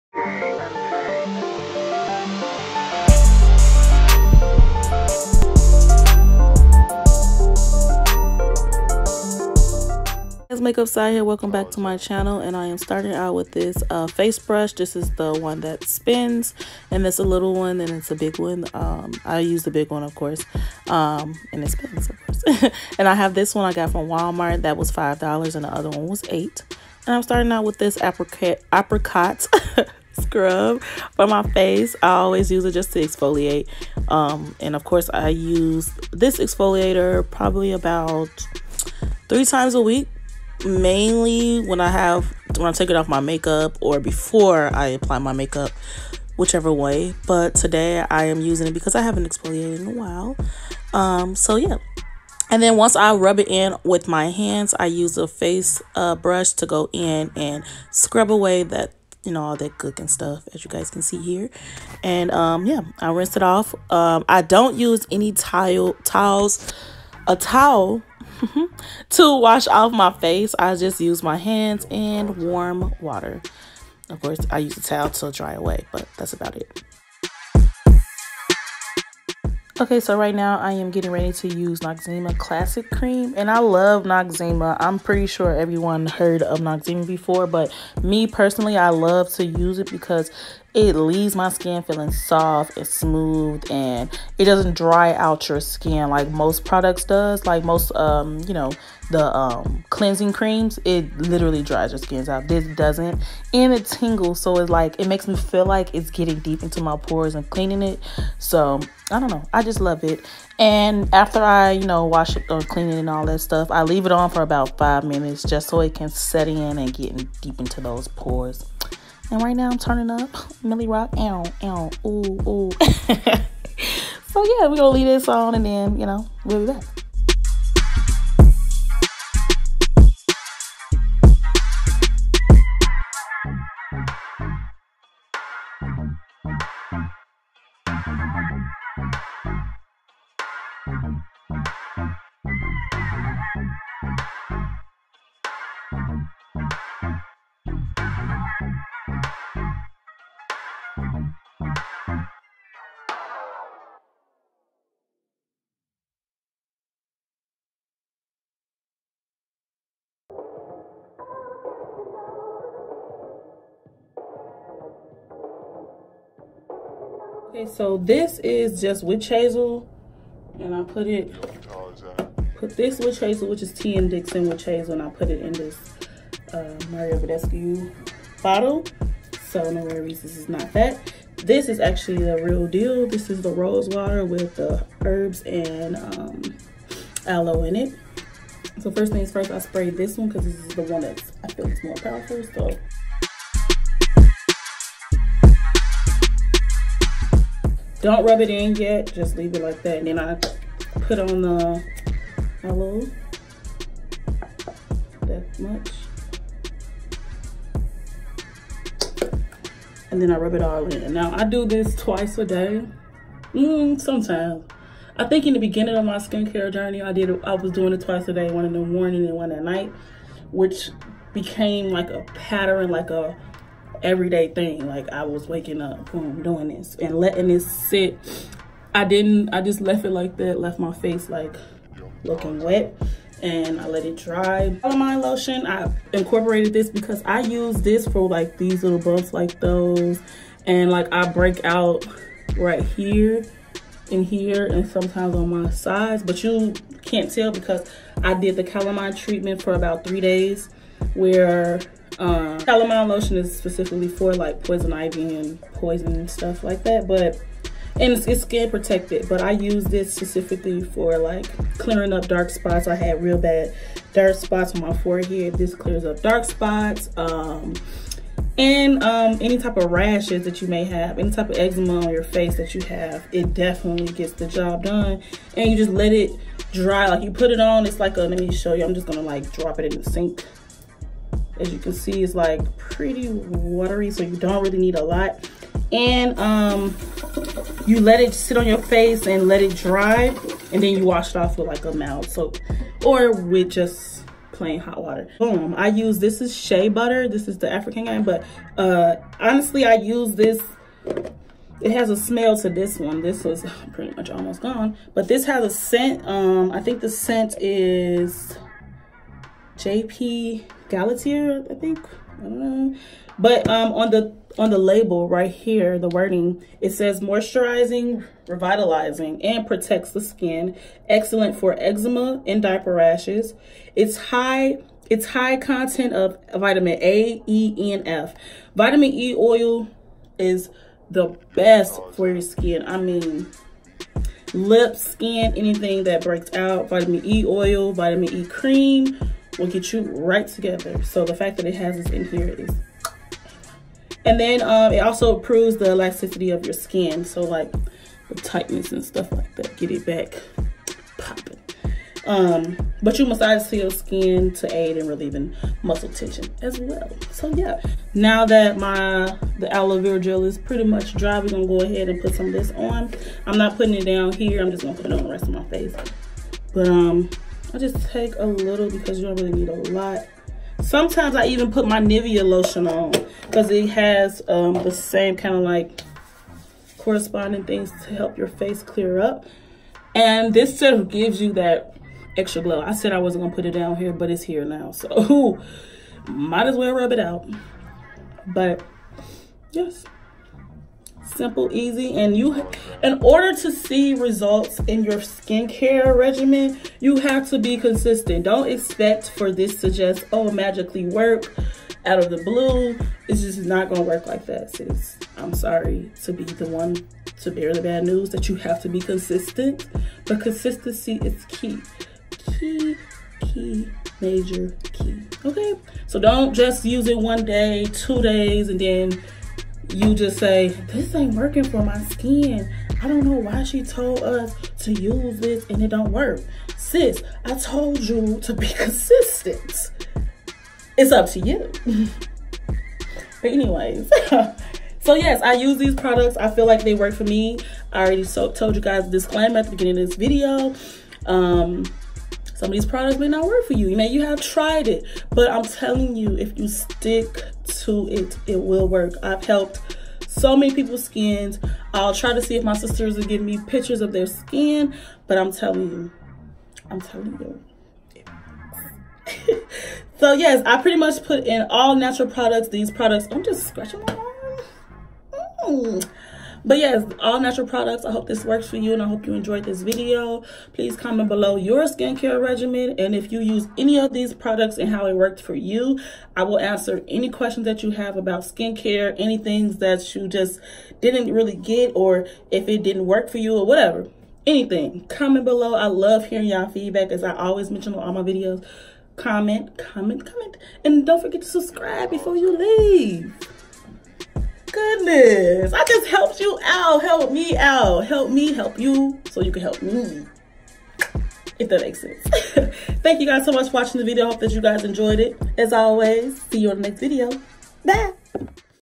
March Madness: Updated Skincare Routine | Combination Skin | 2019
I hope that this video was informative and was helpful for those that struggle with their skin. I have combination skin.. my Tee-zone gets oily and the other half is dry lol I’ve never had skin problems UNTIL I started wearing makeup 😩 so taking care of my skin after all the makeup is washed away is VERY important! But yes I really hope this can/will help with your skin.. if you try this regimen.
God bless 🙏🏾

Noxema
Apricot scrub
Mario burdesco “Rose water”
T.N. Dickinson “Witch hazel”
Tropic Sun “Aloe Vera gel”
African Shea Butter
Calamine lotion

♡Prov. 31:30 (KJV)♡

SC: Syhilove